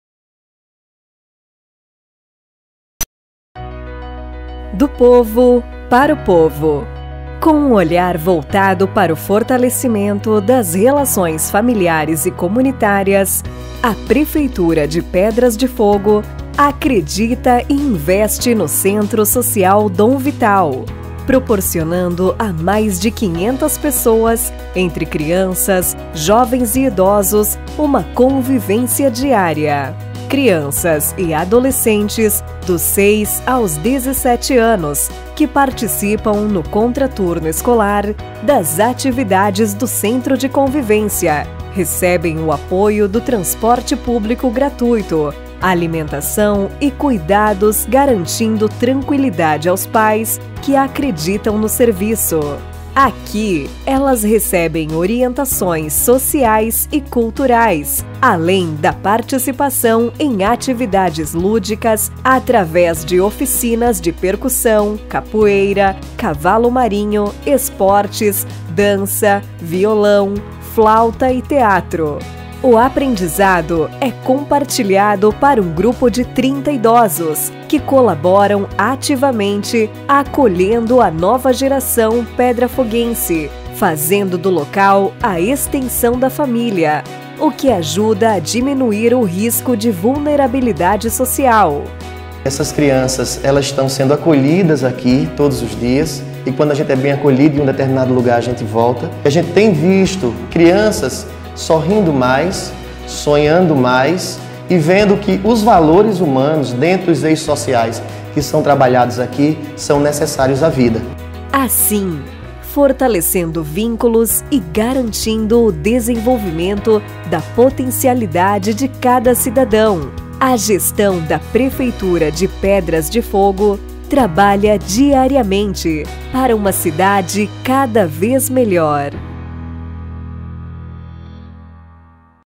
Serviço de Convivência e Fortalecimento de Vínculos
O Serviço de Convivência e Fortalecimento de Vínculos (SCFV) de Pedras de Fogo tem realizado um trabalho de acolhimento e cuidado com as crianças e adolescentes do município, além de acolher os idosos para terem uma socialização plena, afastando-os do ócio negativo.